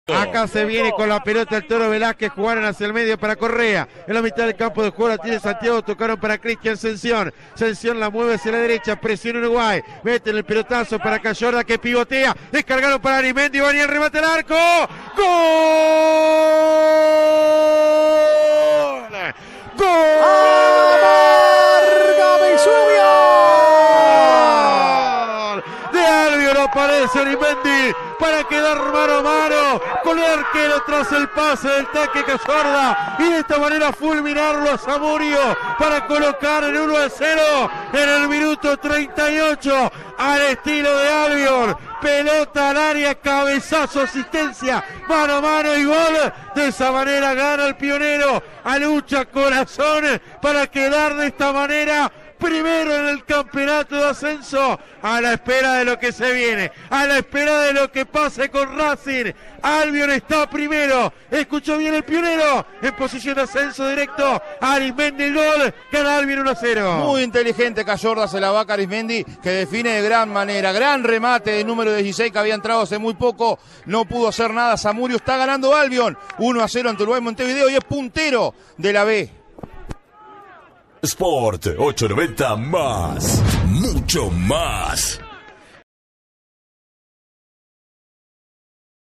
Albion 1 Uruguay Montevideo 0 - Segunda 2021 (por Nicolás Pirri)
El gol del partido, por Nicolás Pirri y Sebastián Goicoechea para Sport 890 Más.